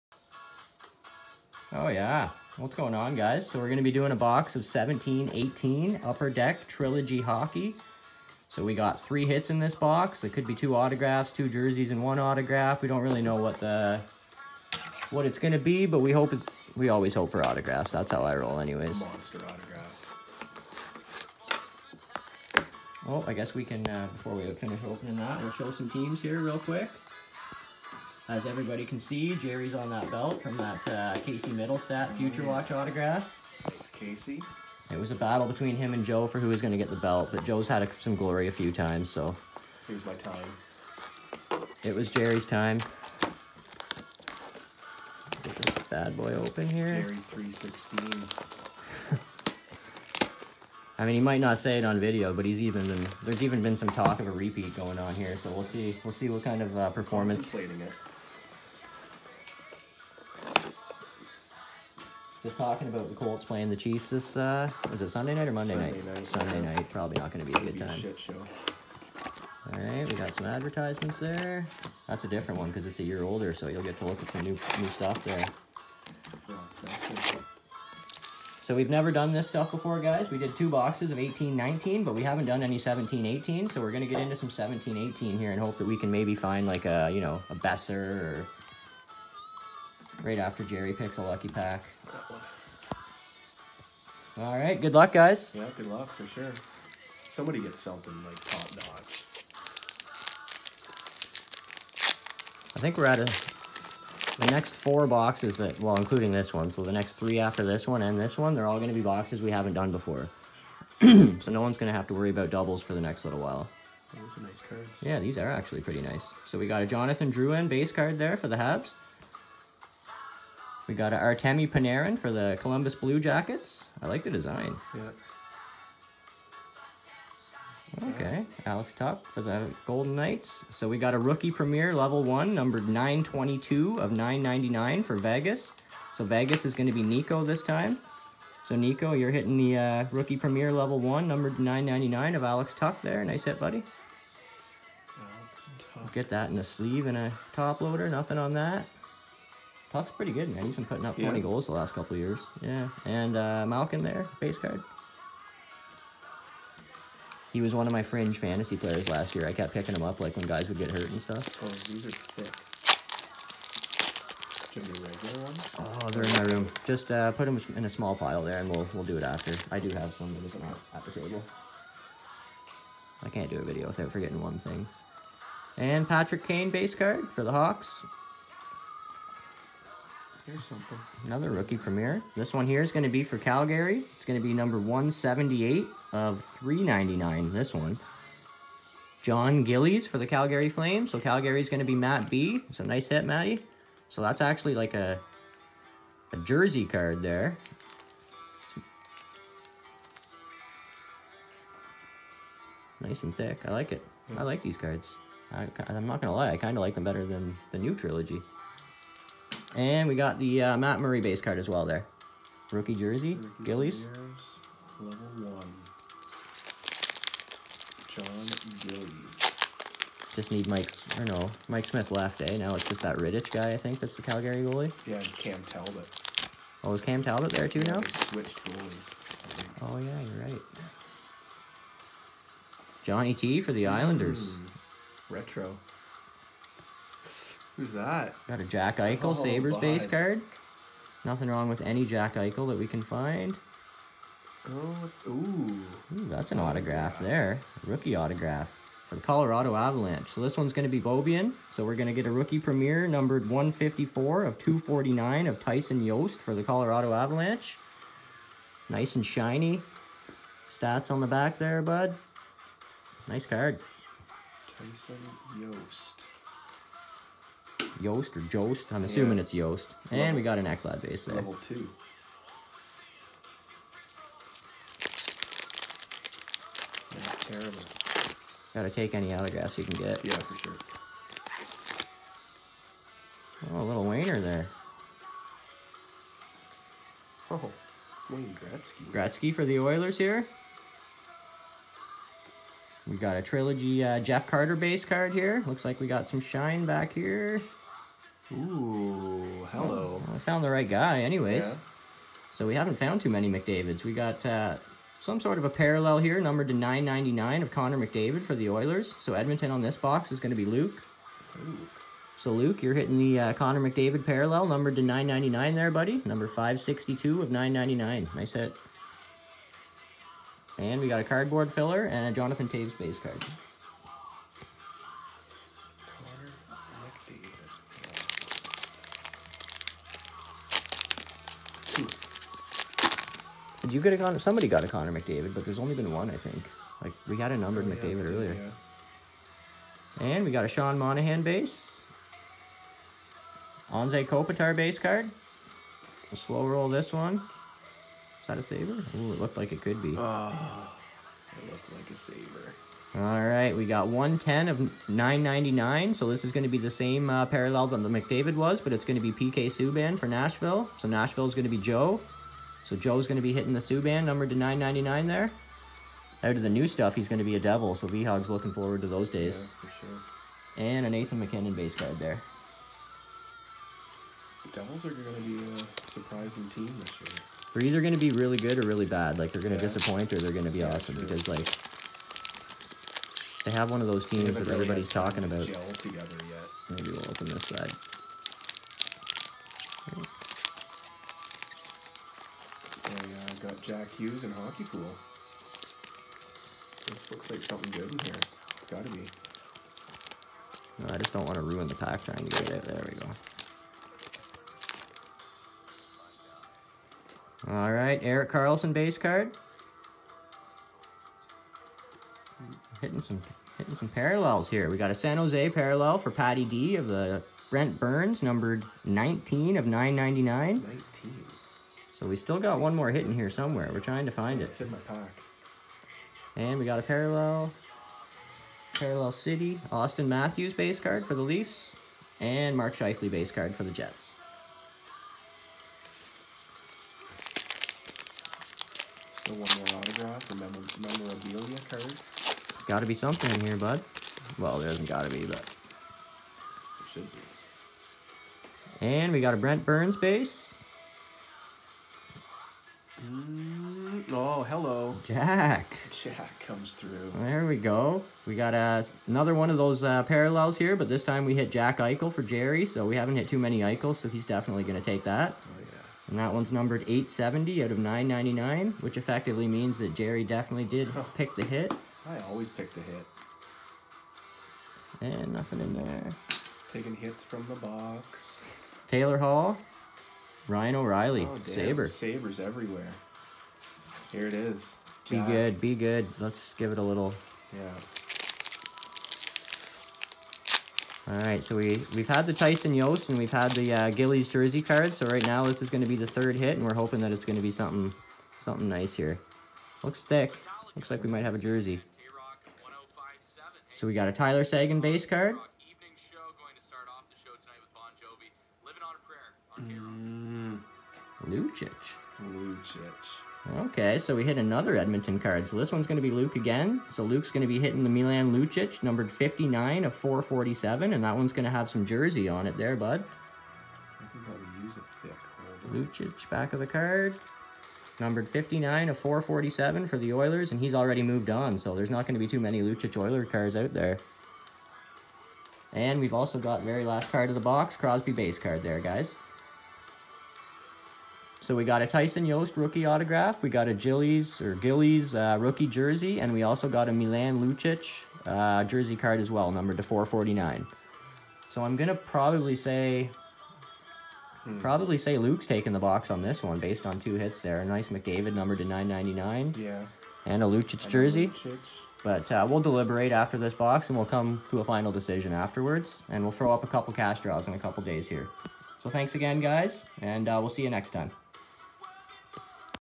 17-18 Upper Deck Trilogy Hockey Hobby Box Break!
17-18 Upper Deck Trilogy Hockey Hobby Box. 8 packs per box, 3 hits (autographs or memorabilia cards). 👍